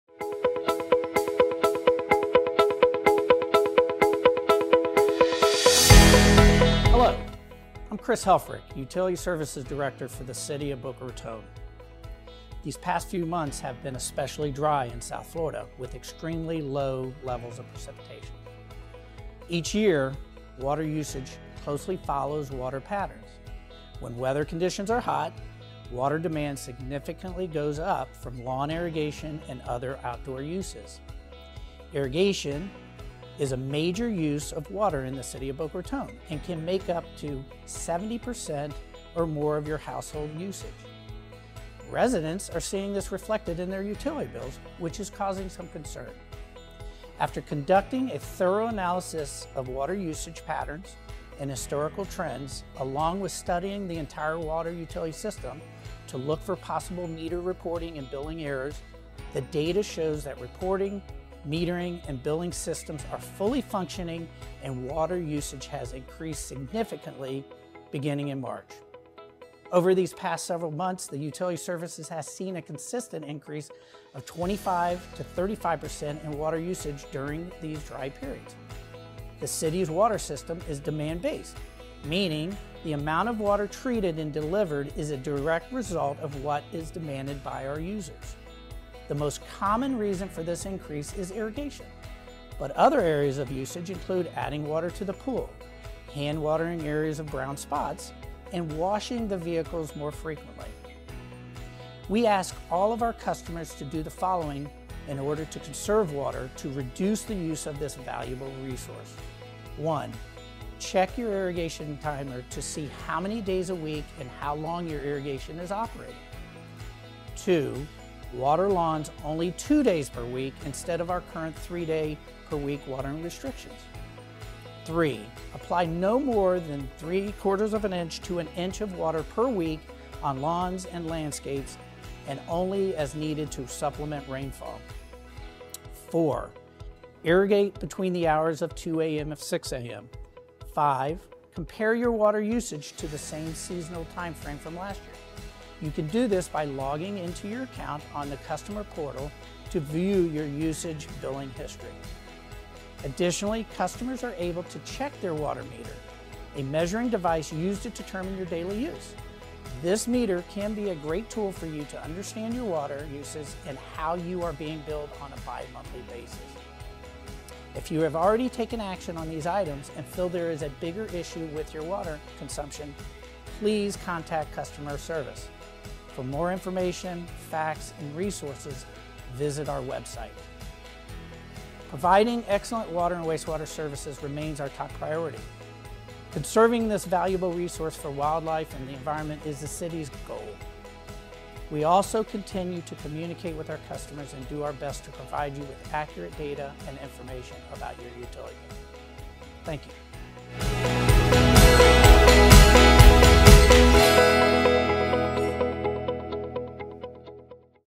Understanding Water Conservation & Usage
Irrigation is a major use of water in the City of Boca Raton and can make up to 70% or more of household usage. Each year, water usage closely follows weather patterns: when weather conditions are hot and dry, water demand significantly increases because lawn irrigation and other outdoor uses increase. The City’s water system is demand-based, meaning the amount of water treated and delivered is a direct result of what is demanded by users.

Because weather conditions have been extremely dry for the past three to four months, there is a need to irrigate lawns more and residents are seeing this reflected in their utility bills. This increase in water use is causing some concern. The data shows our reporting, metering and billing systems are fully functioning, and water usage has indeed increased significantly since the beginning of March. Over these past several months the Utility Services Department has seen a consistent increase of 25-35% in water usage during these dry periods.

As we continue to navigate this dry period together, we ask residents to be mindful of their usage and help in our conservation efforts. Learn more from Utility Services Director, Chris Helfrich or visit myboca.us.